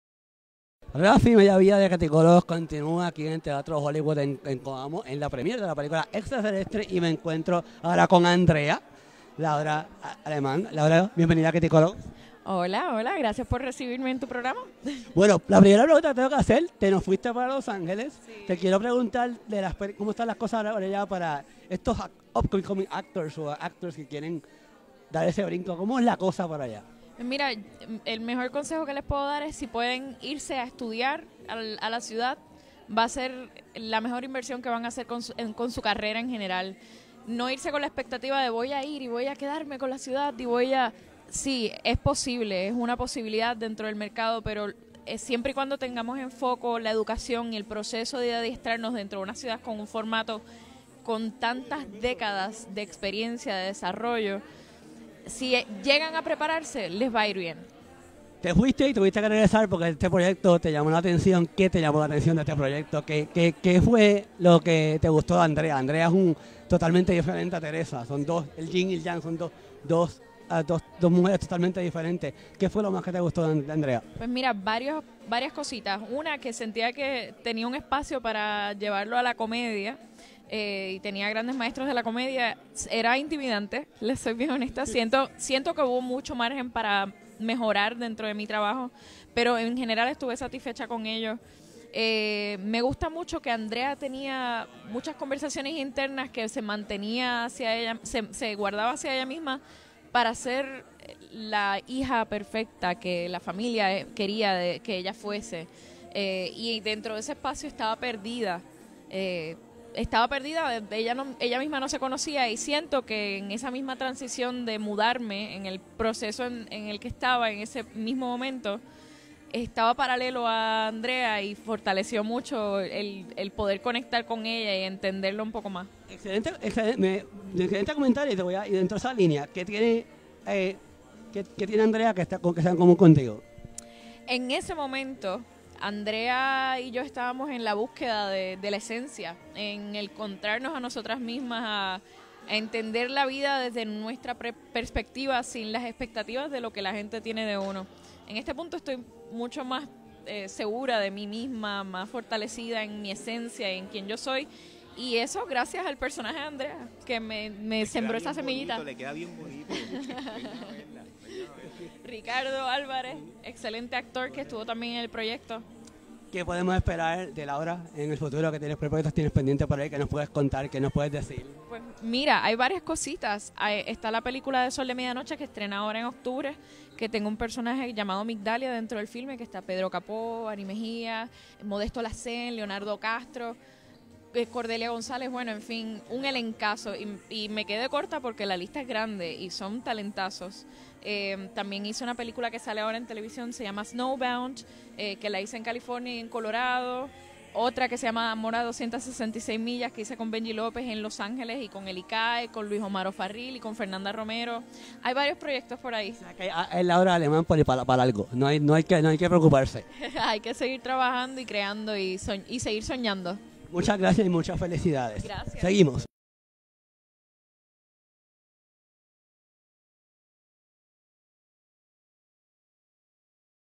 Entrevista con Laura Aleman / Co-protoganista de "Extra Terrestres".
Nuestra entrevista con la actriz Laura Aleman (Laura Aleman Page - @AlemanLaura), co-protagonista de la película boricua, "Extra Terrestres".